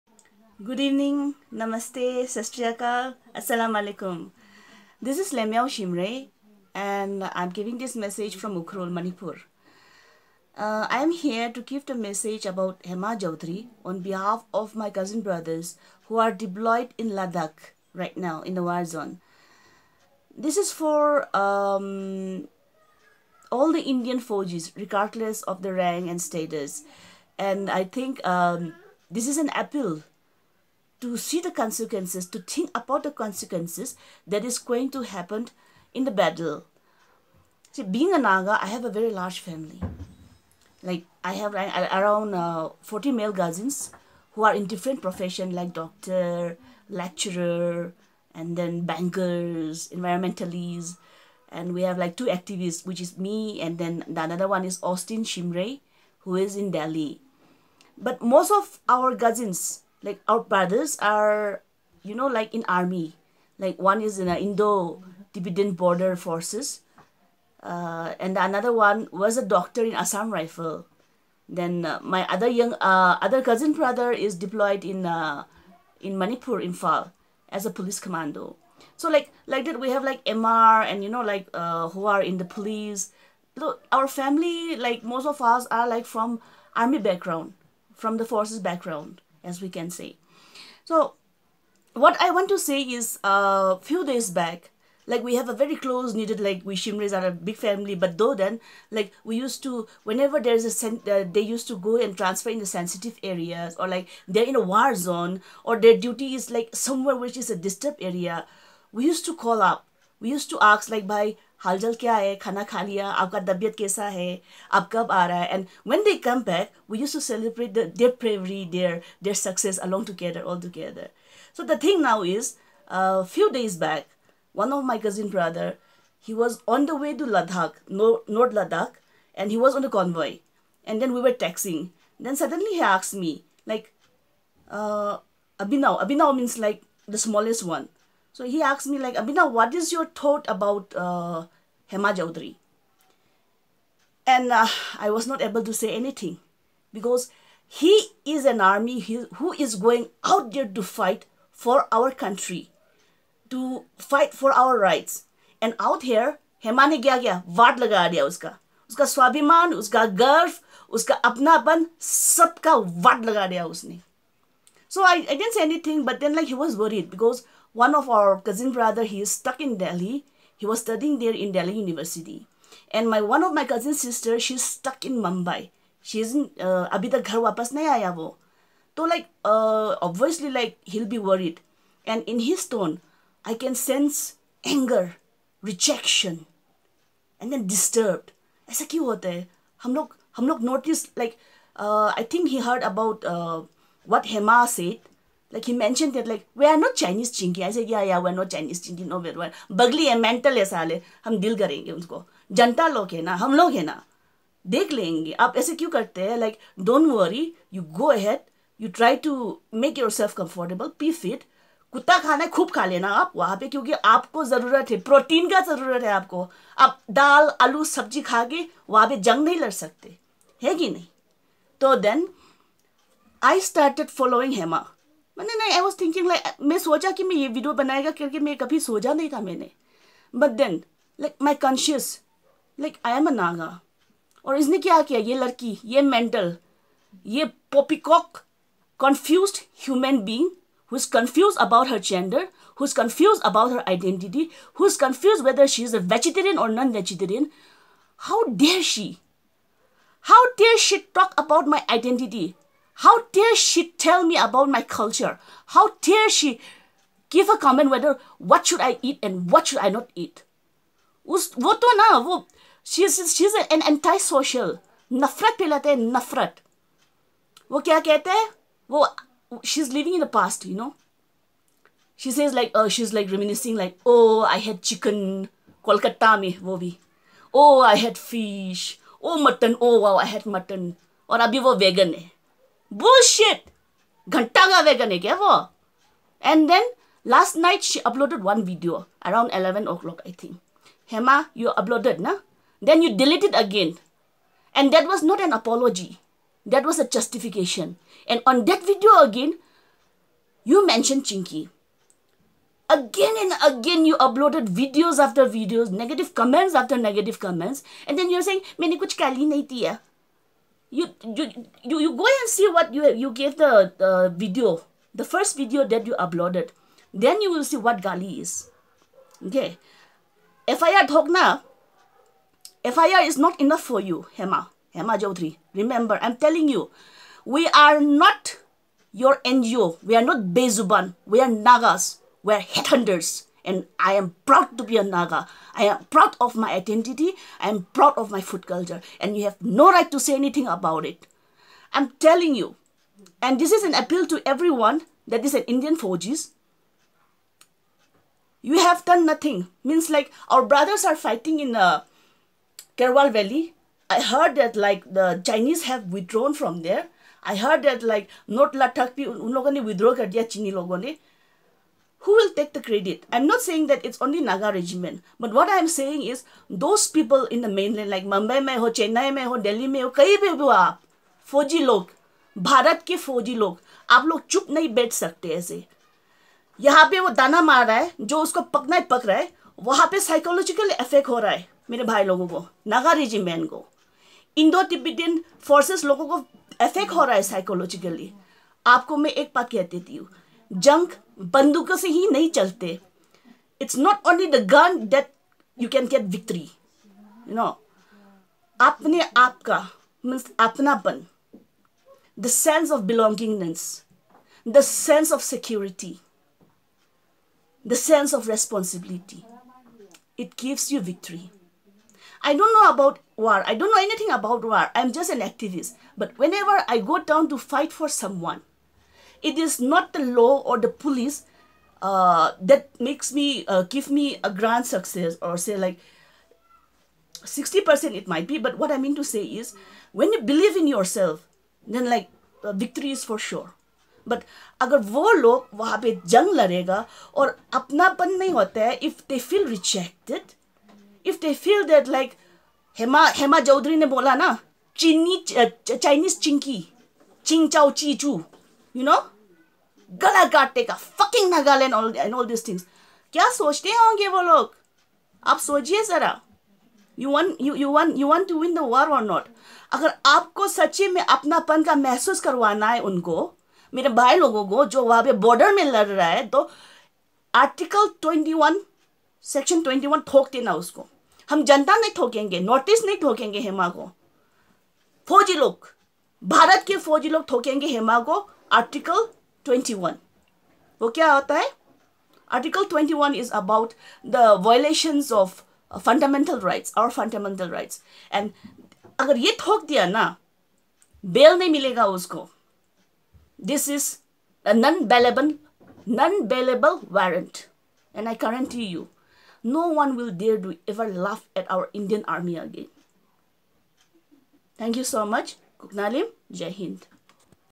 Lemyao Shimray vow not to rest untill Hema Chowdhary is put behind the bar with non bailable warrant
" Finally a Naga lady speaks up against Hema Chowdhary and make an appeal to Indian Army to book Hema Chowdhary under Article 21 for robbing the fundamental Rights of Naga Regiments who are fighting for India at present with Chinese soldiers.